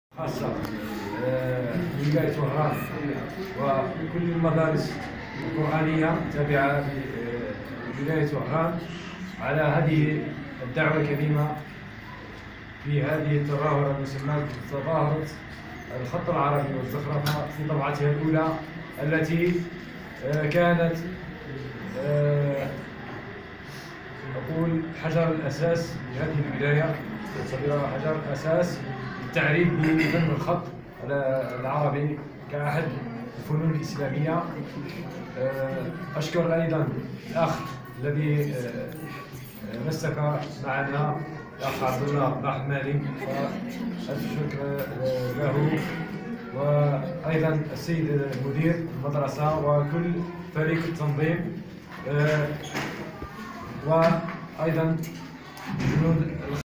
من مداخلتي بتظاهرة الخط العربي و الزخرفة بولاية وهران.. من تنظيم المدرسة العلمية الخاصة
الخطاط زهير بن شعبان